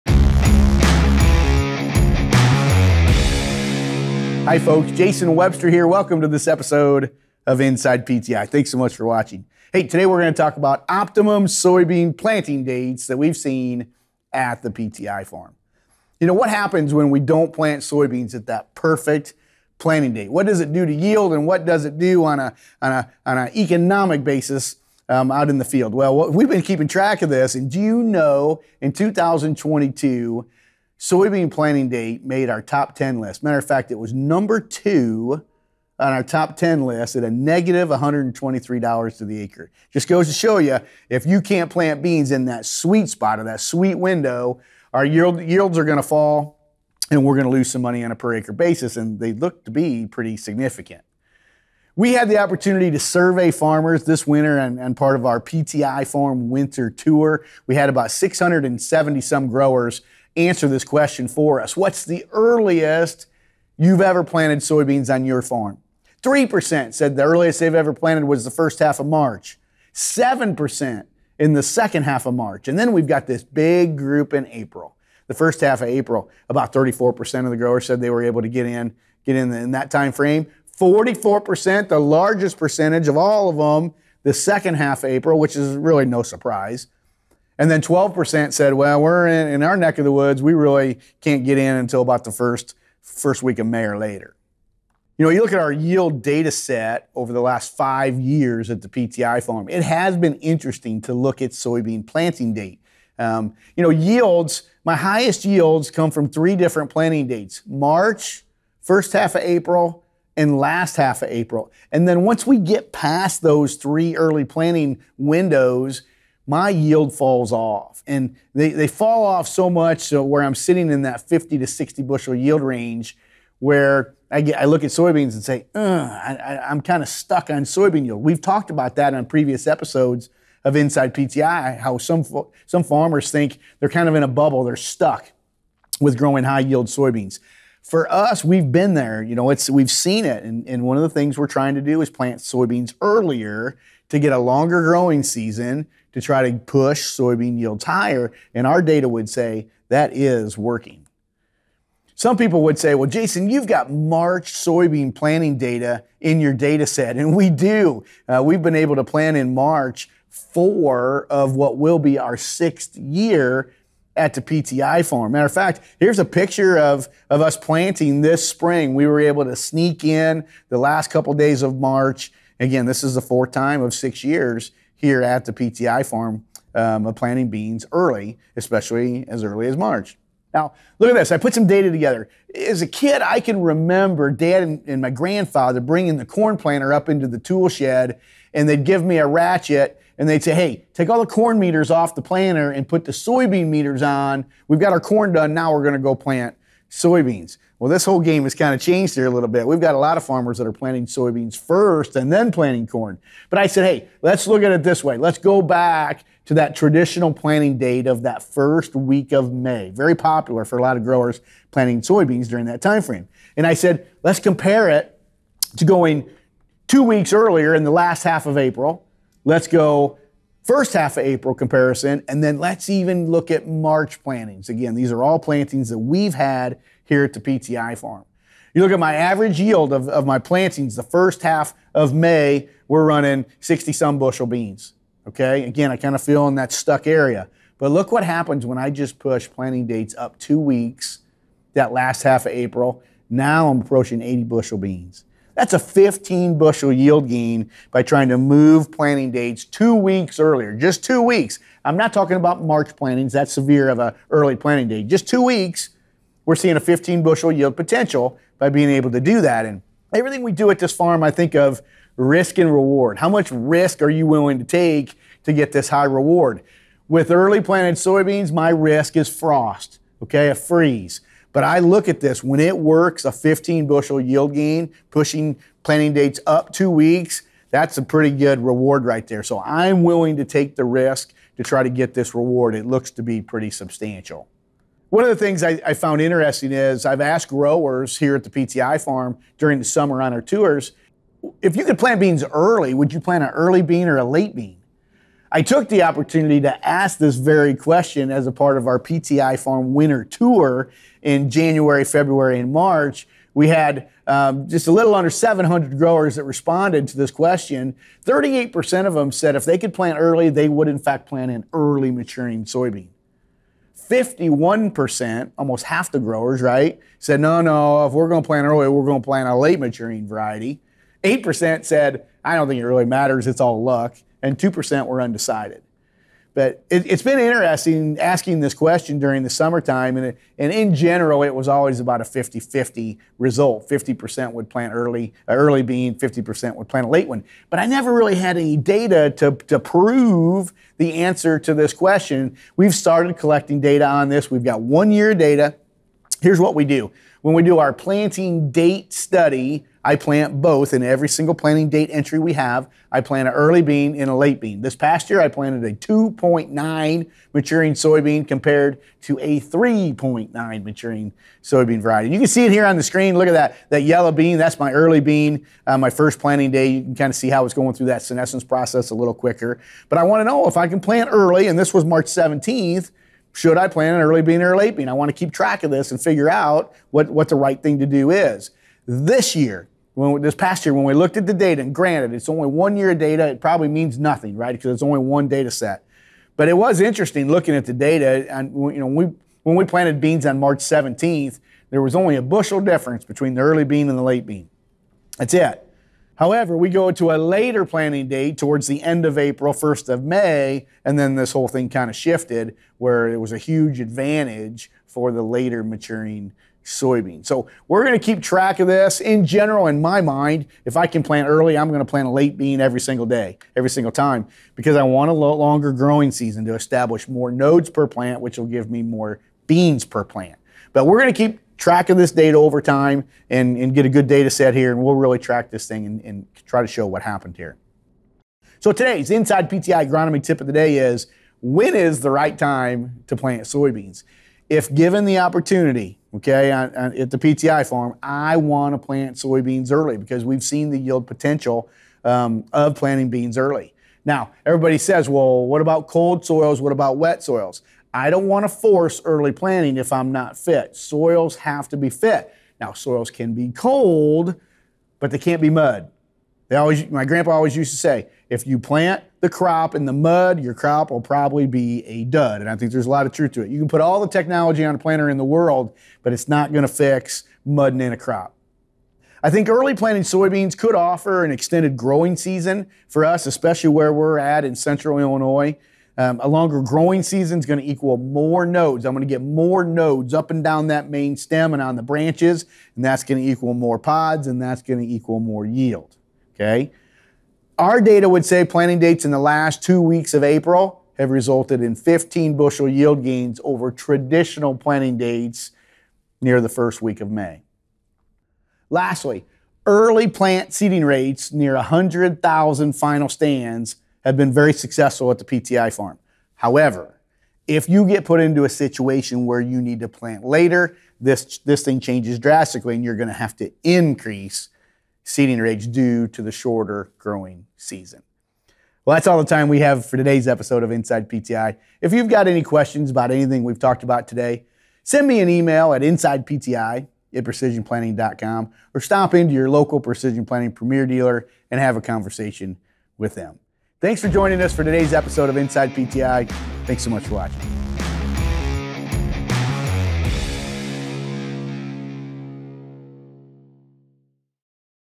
InsidePTI S4•E04 | Optimum Planting Date ‣ Soybean
The window for planting season is upon us! On today's episode, Jason is breaking down the yield we've seen for soybeans in past years based on planting date throughout the season.